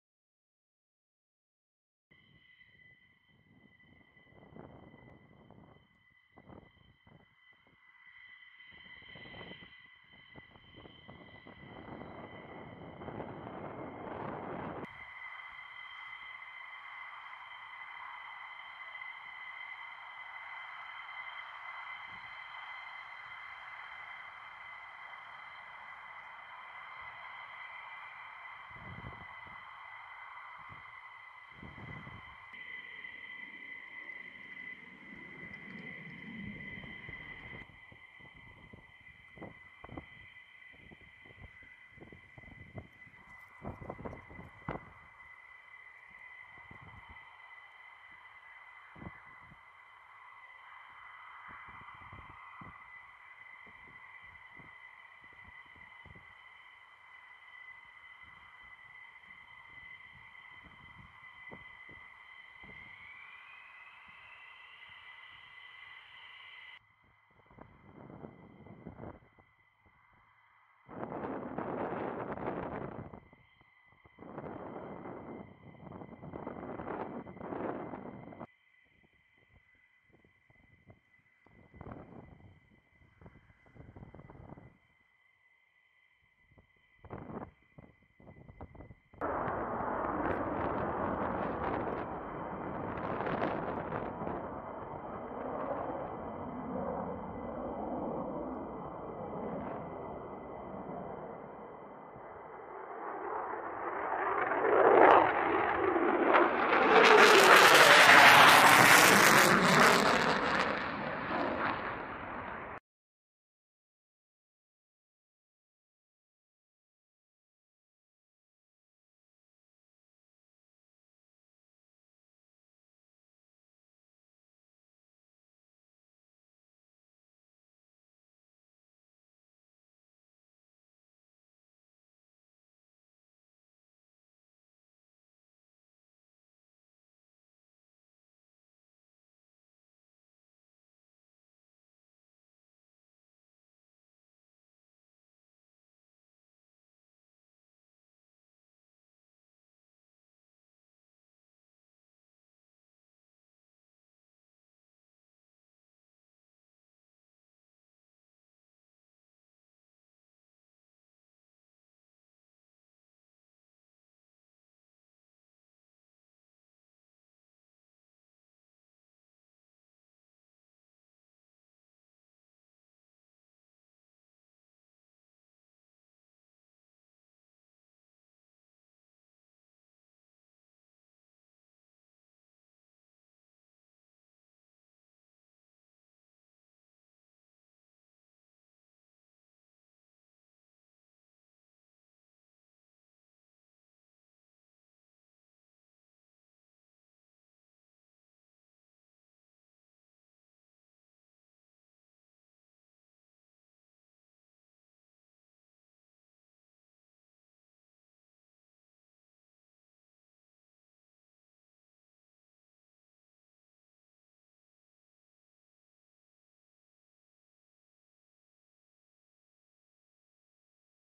F-15E Strike Eagles conduct routine flying ops at RAF Lakenheath
BRANDON, SFK, UNITED KINGDOM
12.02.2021

The 48th Fighter Wing conducts routine flying operations at Royal Air Force Lakenheath, England, Feb. 12, 2021. The Liberty Wing is postured to provide worldwide responsive combat airpower in support of its allies and partners across a wide spectrum of military operations. (U.S. Air Force video by Staff Sgt. Rachel Maxwell)

Starting at 00:01:53, B-Roll includes aircraft takeoff and maneuvers recorded in slow motion (no audio available).

Film Credits: Video by Staff Sgt. Rachel Maxwell
48th Fighter Wing Public Affairs